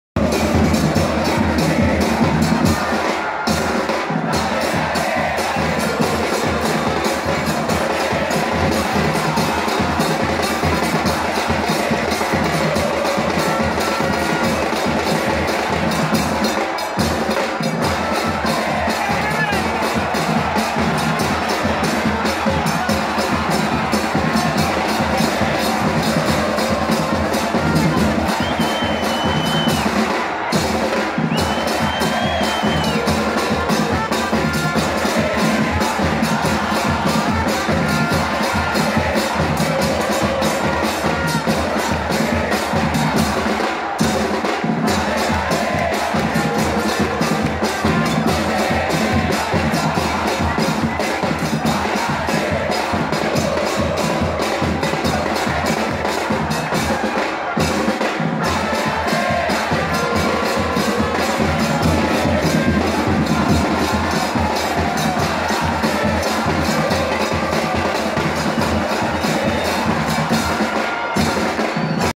VIDEO BARON ROJO SUR SALIDA CONTRA RIO NEGRO /BOMBAS DE HUMO/ EN LA TRIBUNA SUR
Vídeos , Noticias, partidos  Y Reportajes del america de cali
El aguante de la banda del diablo y   los cantos del Barón Rojo Sur